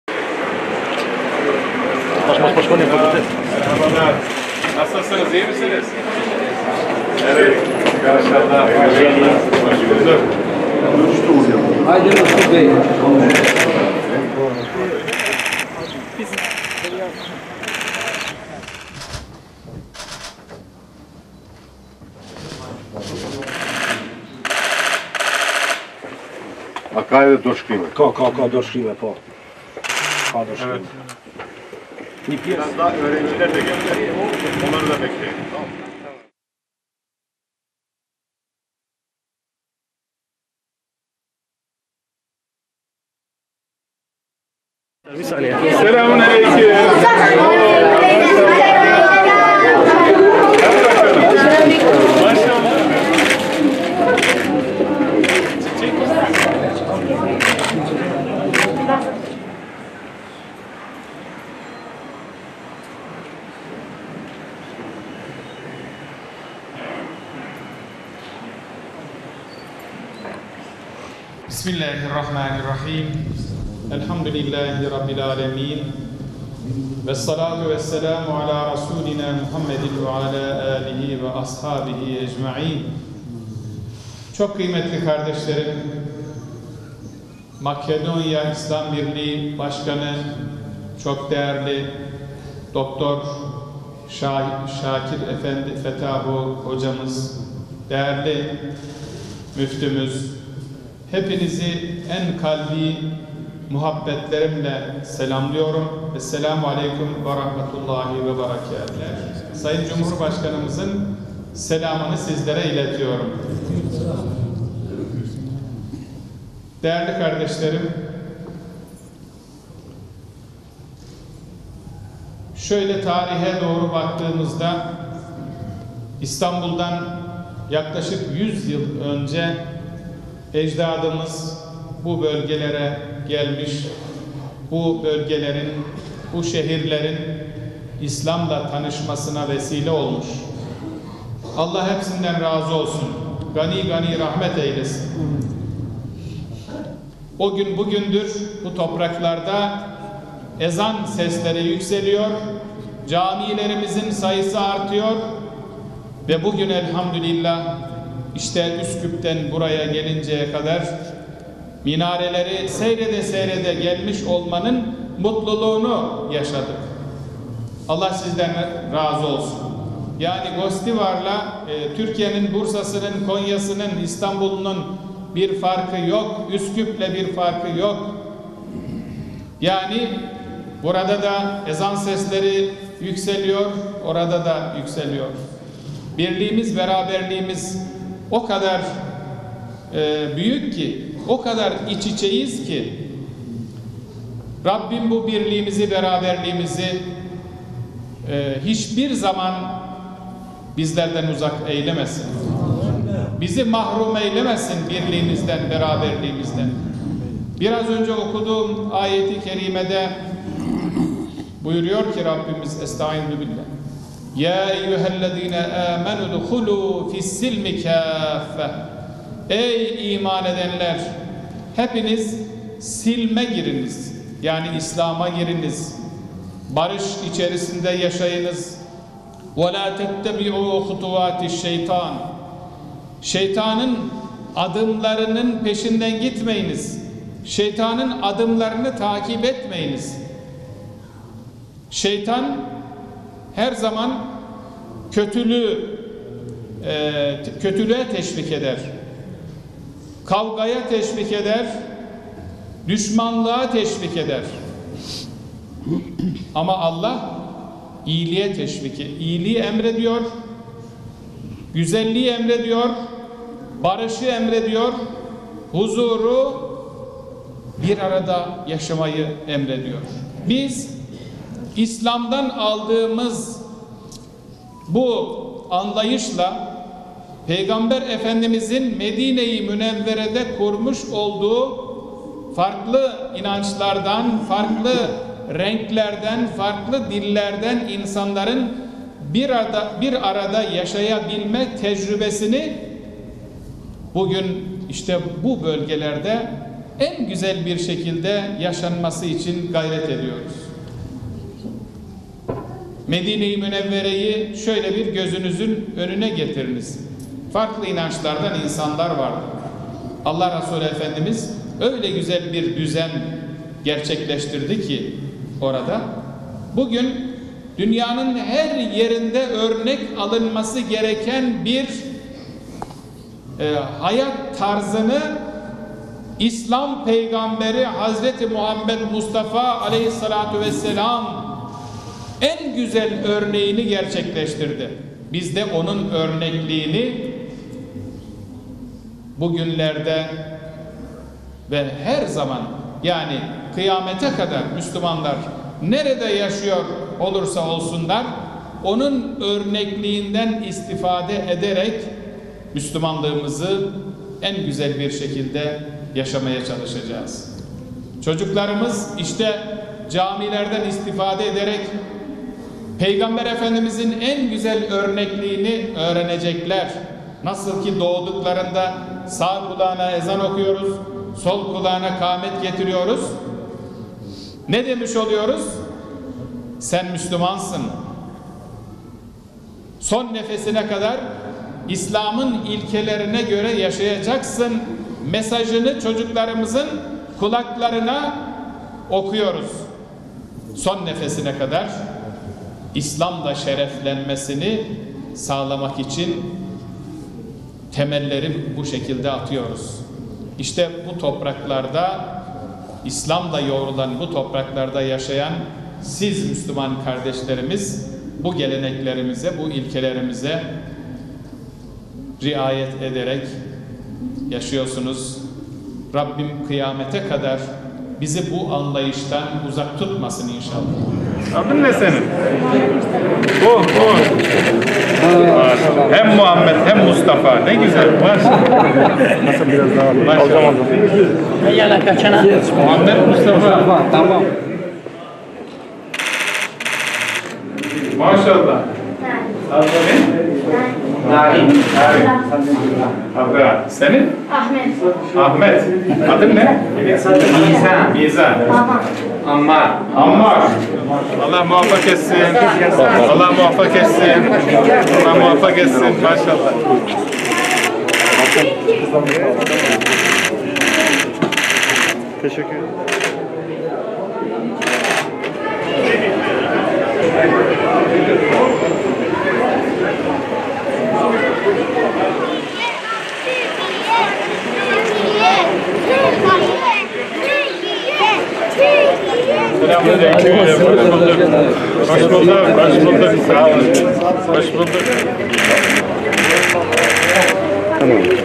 Diyanet İşleri Başkanı Erbaş, Gostivarlılarla bir araya geldi
Makedonya’da Gostivarlılara Saat Camii’nde hitap eden Diyanet İşleri Başkanı Prof. Dr. Ali Erbaş, Balkanların İstanbul’dan yaklaşık yüzyıl önce İslam’la tanıştığını hatırlatarak “O gün bugündür bu topraklarda ezan sesleri yükseliyor. Allah sizlerden razı olsun.” dedi